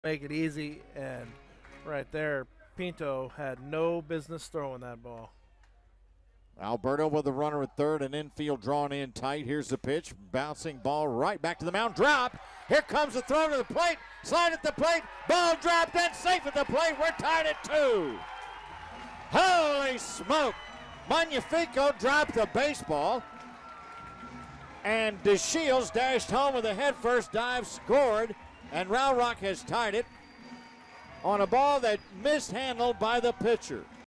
Round Rock's Alberto plates run with a dribbler
6/27/16: Round Rock's Hanser Alberto plates the game-tying run with a dribbler back to the mound in the 12th against Colorado Springs